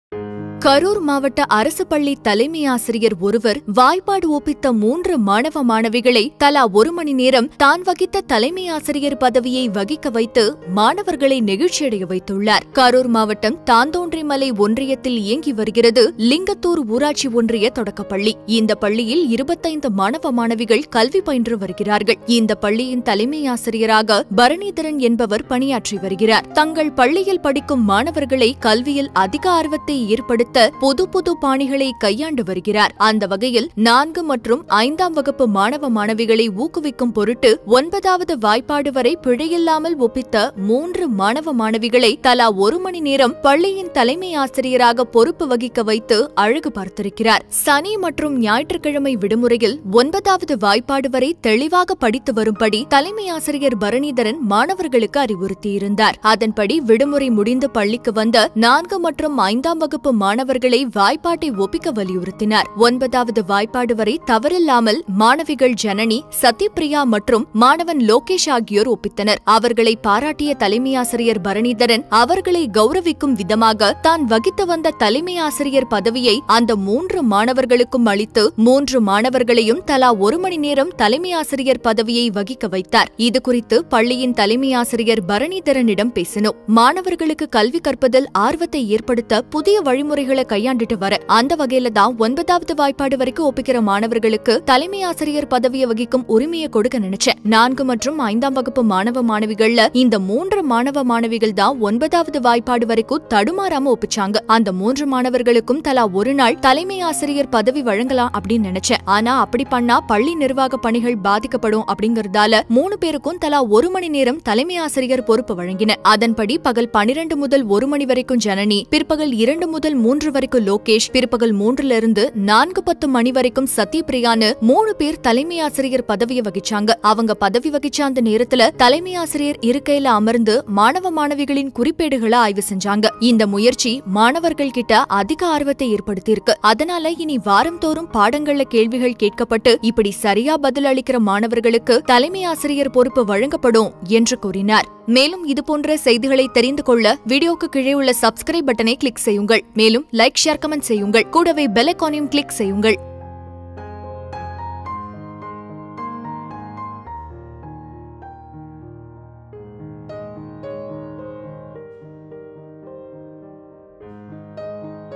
சரியா பதில் சொன்னால் தலைமையாசிரியர் பதவி...கரூர் அரசுப் பள்ளி ஆச்சர்யம்
இனி வாரம்தோறும் பாடங்களில் கேள்விகள் கேட்கப்பட்டு இப்படி சரியாக பதில் அளிக்கும் மாணவர்களுக்கு தலைமை ஆசிரியர் பொறுப்பு வழங்கப்படும்.

Reporter - துரை.வேம்பையன்